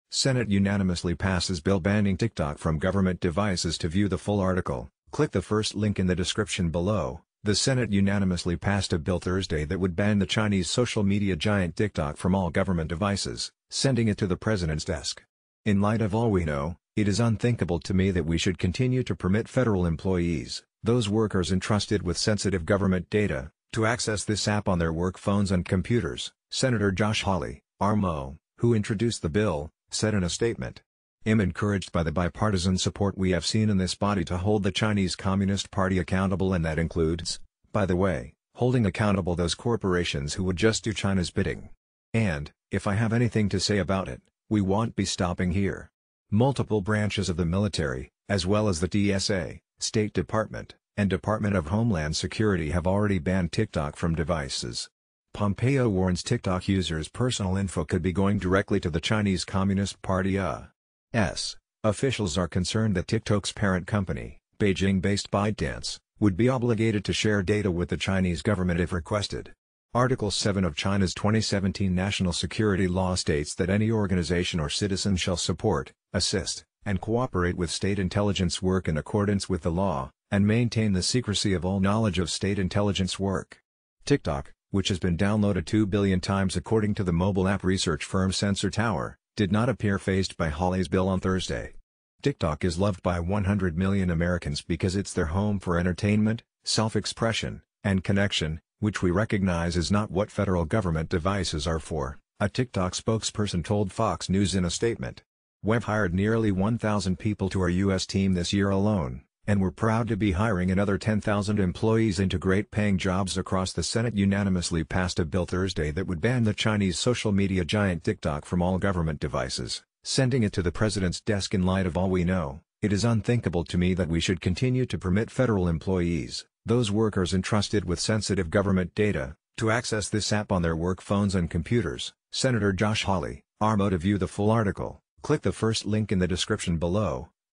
Senate unanimously passes bill banning TikTok from government devices
The Senate unanimously passed a bill Thursday that would ban the Chinese social media giant TikTok from all government devices, sending it to the presidents desk. In light of all we know, it is unthinkable to me that we should continue to permit federal employees, those workers entrusted with sensitive government data, to access this app on their work phones and computers, Senator Josh Hawley, R-Mo. , who introduced the bill, said in a statement.  Im encouraged by the bipartisan support we have seen in this body to hold the Chinese Communist Party accountable and that includes, by the way, holding accountable those corporations who would just do Chinas bidding.  And, if I have anything to say about it, we wont be stopping here. Multiple branches of the military, as well as the TSA, State Department, and Department of Homeland Security have already banned TikTok from devices. POMPEO WARNS TIKTOK USERS PERSONAL INFO COULD BE GOING DIRECTLY TO THE CHINESE COMMUNIST PARTYU. S.  officials are concerned that TikToks parent company, Beijing-based Bytedance, would be obligated to share data with the Chinese government if requested. Article 7of Chinas 2017 national security law states that any organization or citizen shall support, assist, and cooperate with state intelligence work in accordance with the law, and maintain the secrecy of all knowledge of state intelligence work. TikTok, which has been downloaded two billion times according to the mobile app research firm Sensor Tower, did not appear phased by Hawleys bill on Thursday. "TikTok is loved by 100 million Americans because its their home for entertainment, self-expression, and connection, which we recognize is not what federal government devices are for, a TikTok spokesperson told Fox News in a statement.  Weve hired nearly 1,000 people to our US team this year alone, and were proud to be hiring another 10,000 employees into great paying jobs across